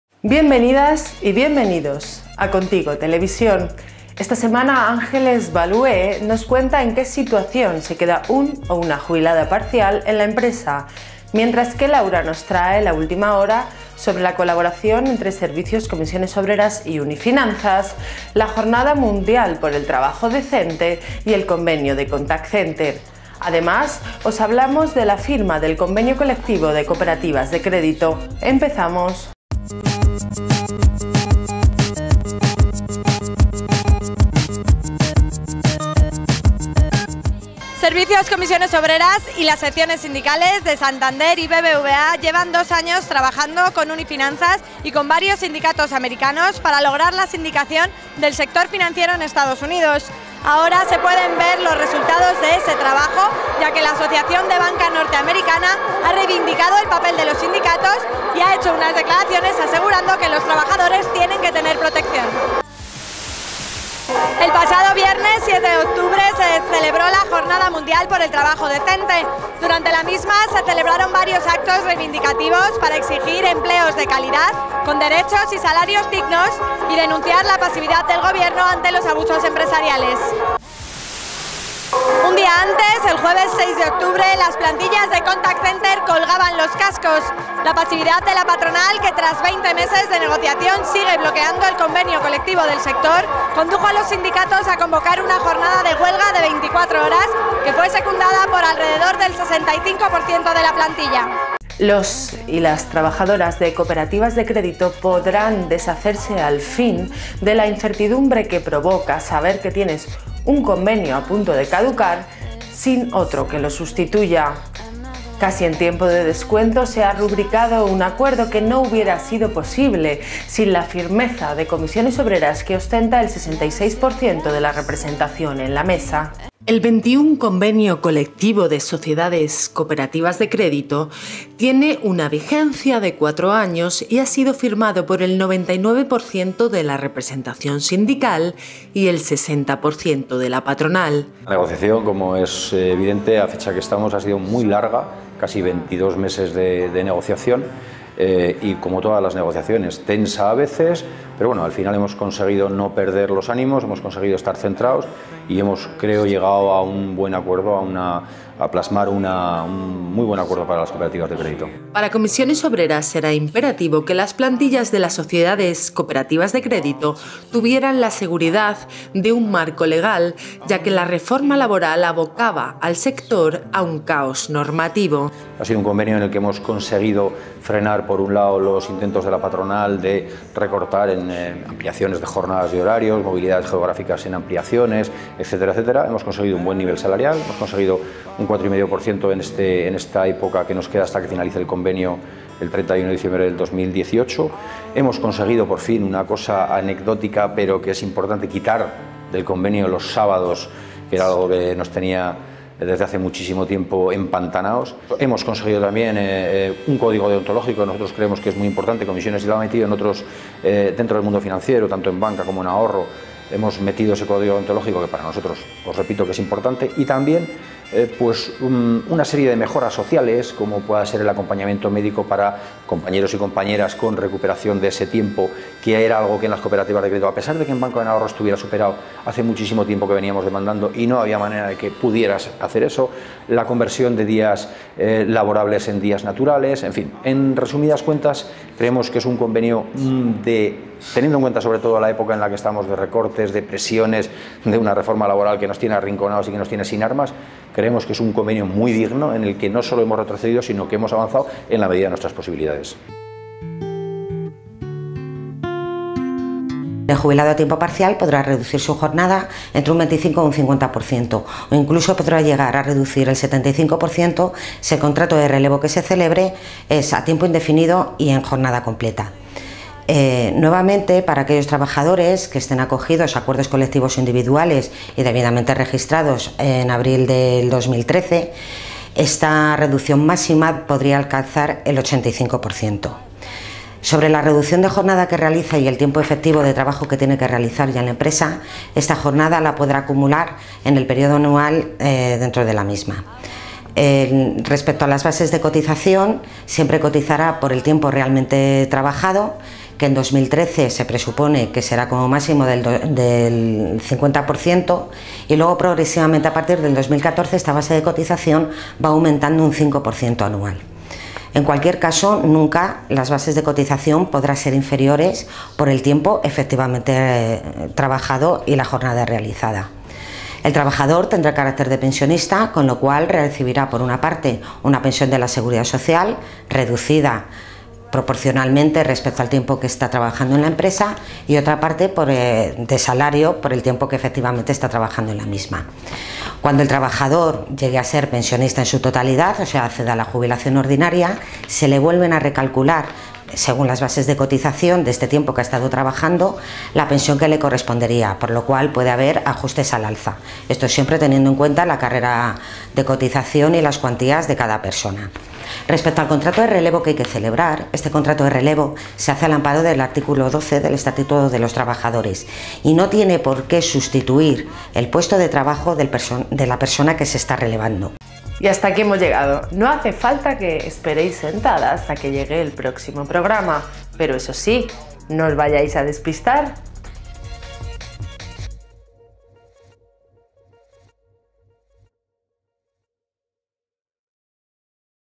Firmado el Convenio de Cooperativas de Crédito
Esta semana Ángeles Balué nos cuenta en qué situación se queda un o una jubilada parcial en la empresa. Mientras, Laura nos trae la última hora sobre la colaboración de Servicios CCOO con UNI Finanzas, la Jornada Mundial por el Trabajo Decente y el Convenio de Contact Center. Además, os hablamos de la firma del Convenio Colectivo de Cooperativas de Crédito.